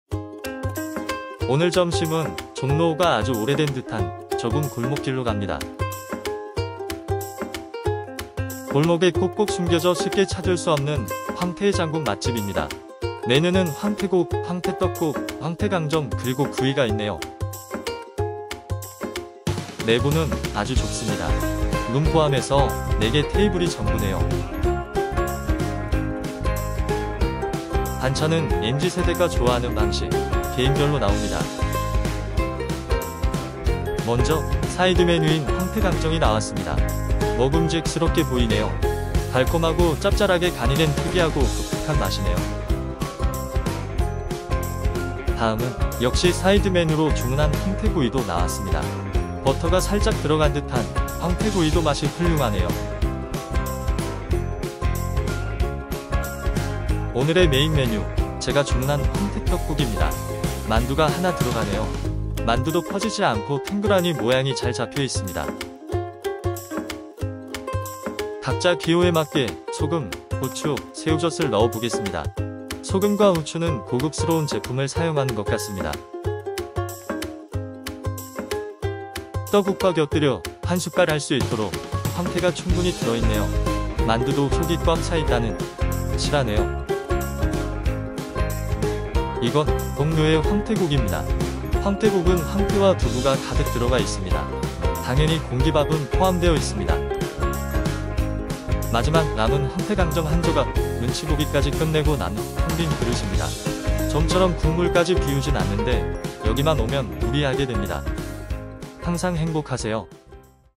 진담황태국 종로5가 해장맛집 Dried Pollack Hangover Soup
* 종로5가와 종로6가 사이 골목에 숨겨진 황태해장국 맛집. 황태해장국, 황태떡국, 황태강정,황태구이.
* Dried Pollack Hangover Soup restaurant hidden in an alley between Jongno 5-ga and Jongno 6-ga. Dried Pollack Hangover Soup, Yellow Pollack Candy, Grilled Pollack